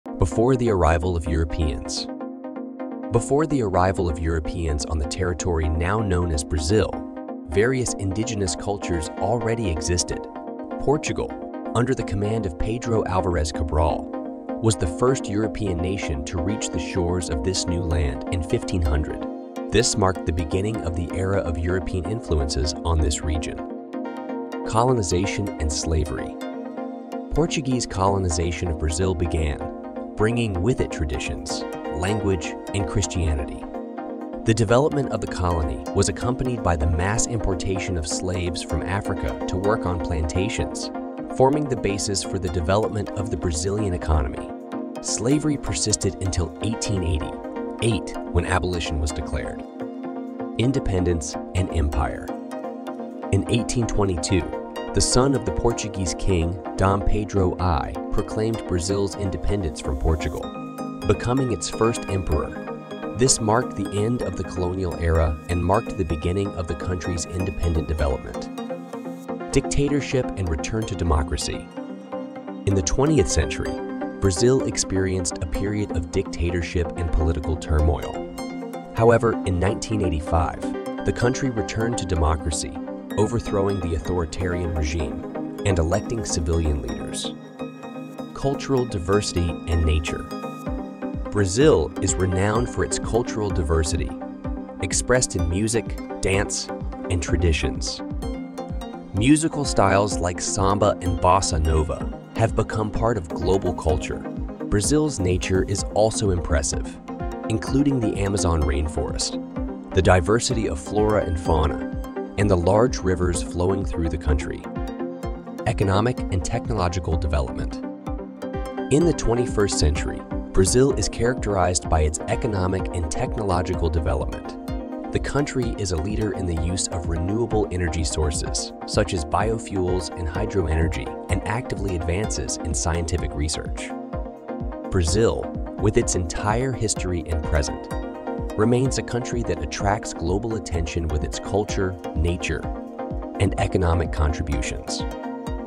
Brazil: From Discovery to Present - Journey Through History
This video is an exhilarating journey through time, unfolding the history of Brazil from its early days of discovery to its contemporary achievements. Embark on a captivating kaleidoscope of events that shaped this vast country and influenced its culture, economy, and society.

From the arrival of Portuguese explorers to the formation of the colony, from the days of the empire to the abolition of slavery—each stage of history is unveiled before you. Learn about influential figures, events, and transformations that have made Brazil what it is today.

Travel through time, unravel the mysteries of the past, and understand how they have impacted the modern image of Brazil. This video is an opportunity to open the doors to a history that significantly shaped the formation of this unique country.